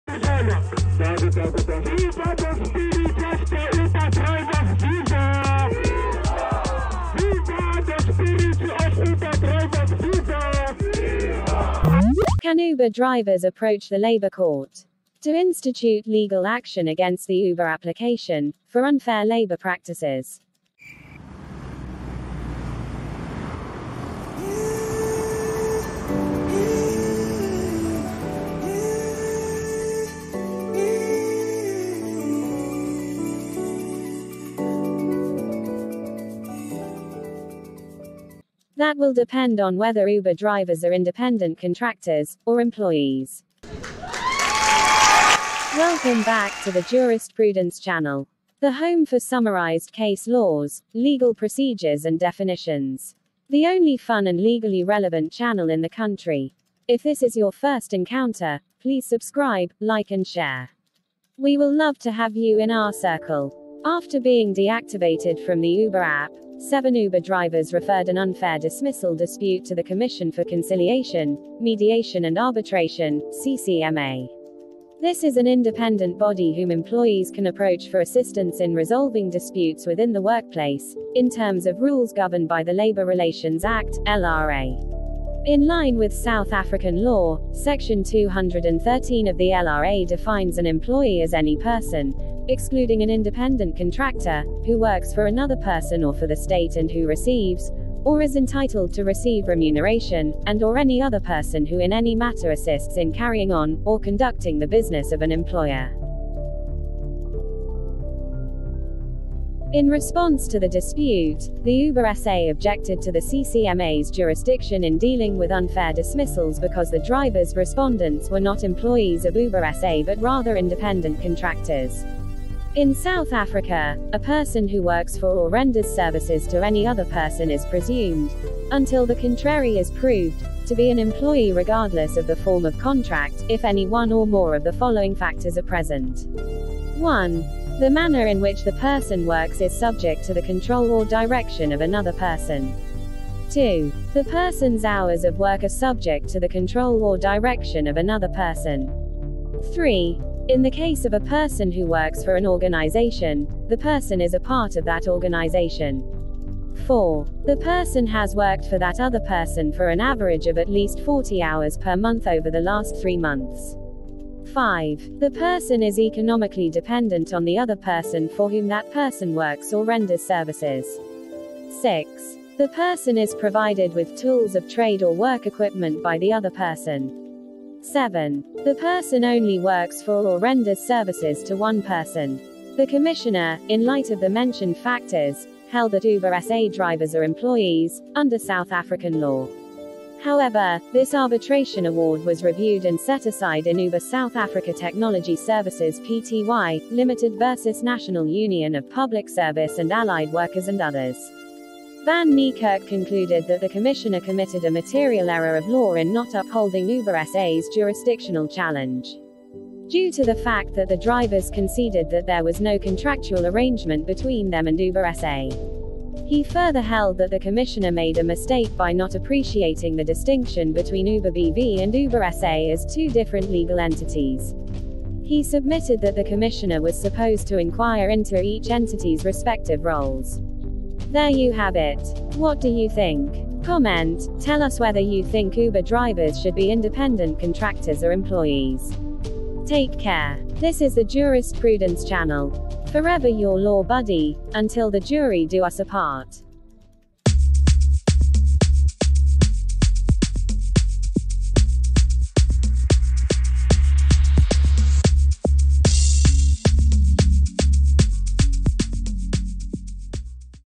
Uber business South Africa |Drivers Being Deactivated |Exploitation |Case Law Summary
Welcome back to the Jurist Prudence Channel!!! ❤️❤️❤️ This is a summary case of Uber South Africa Technology Services (Pty) Ltd v National Union of Public Service and Allied Workers (NUPSAW) and Others (2018). ​

This video further explains what an employee is under the scope of South African law.

We hope you find this useful and informative.
We love Law and you.
_The Jurist Prudence Team ❤️😎